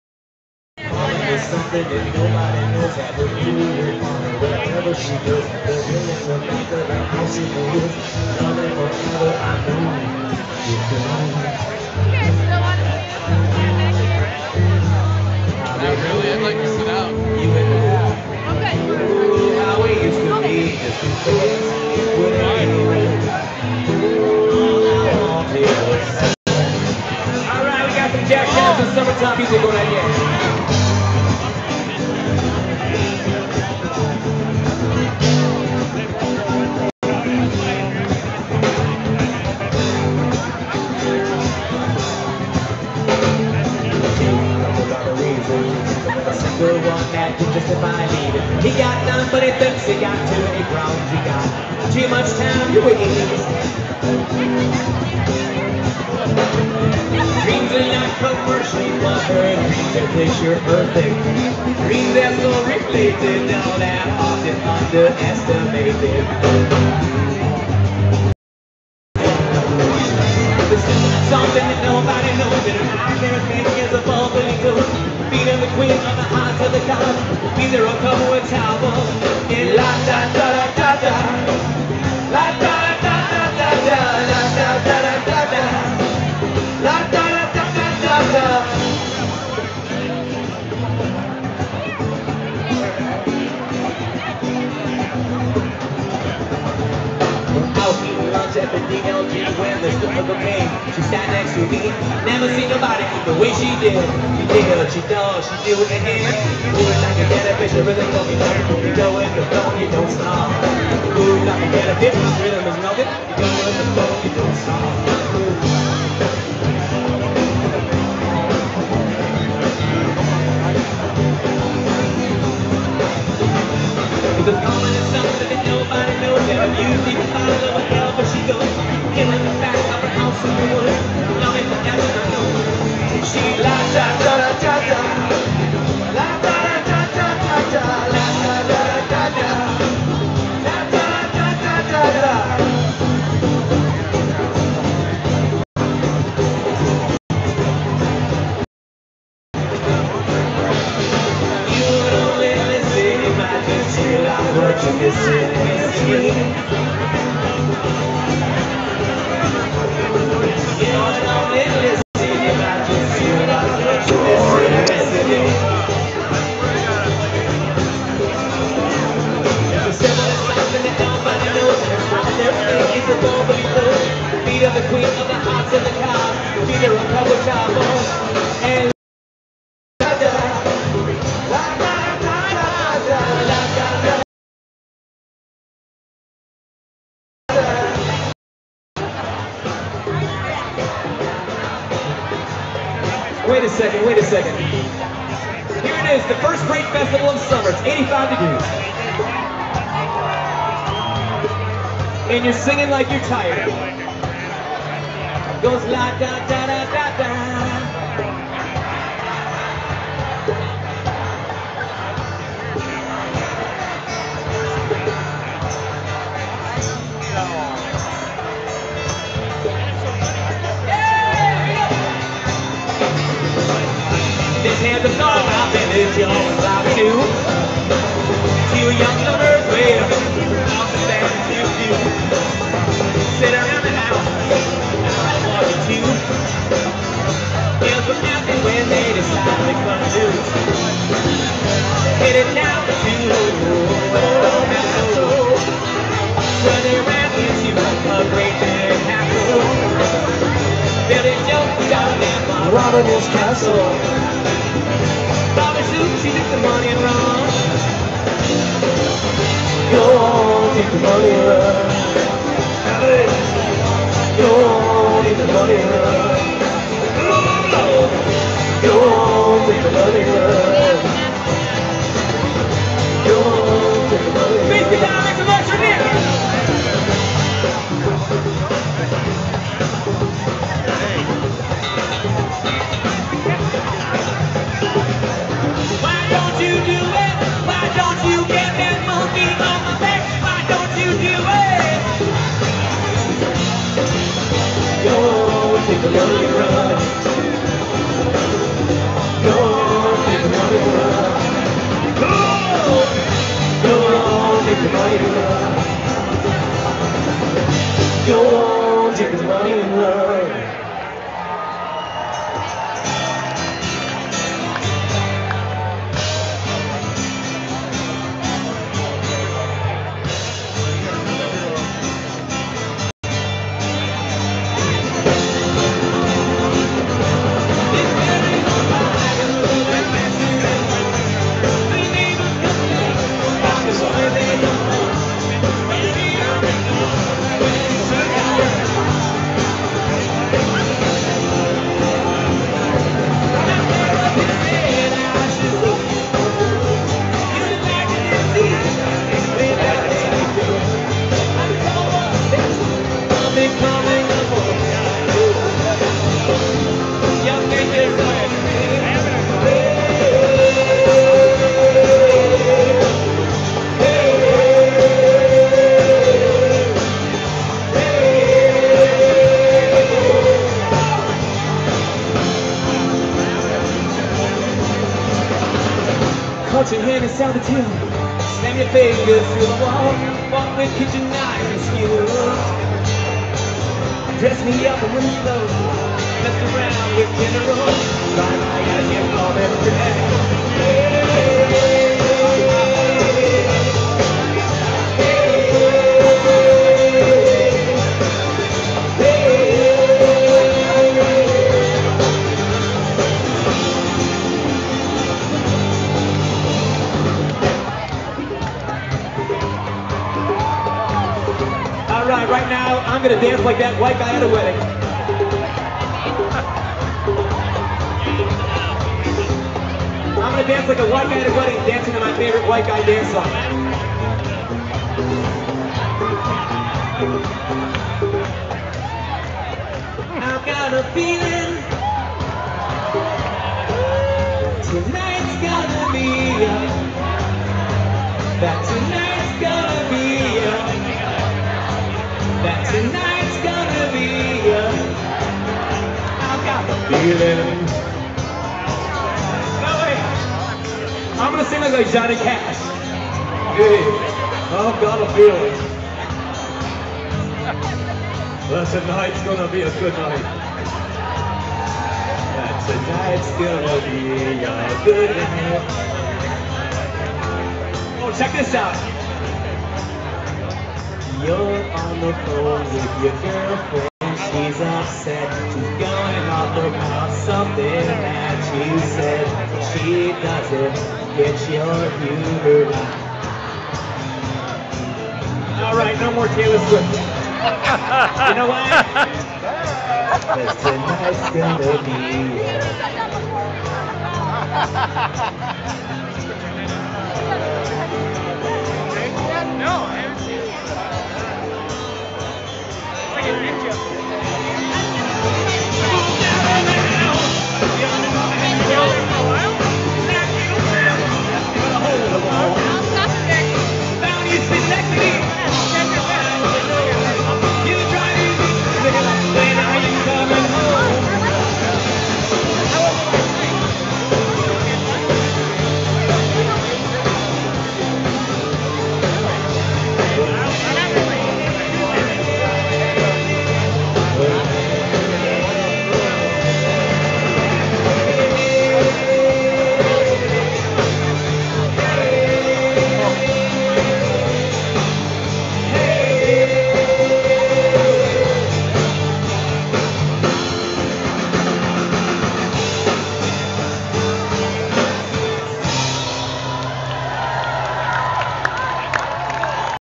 Love Monkies (1) video-2010-05-29-20-28-30.3gp
The Love Monkies from the Brat Fest held at the Alliant Energy Center in Madison, Wisconsin over the Memorial Day weekend.  Another record year for Brat sales in excess of 200,000 brats were sold and consumed at the 2010 Brat Fest.  They had 3 stages with music,  A Midway with many rides was also available along with bounce houses and other kid friendly activities.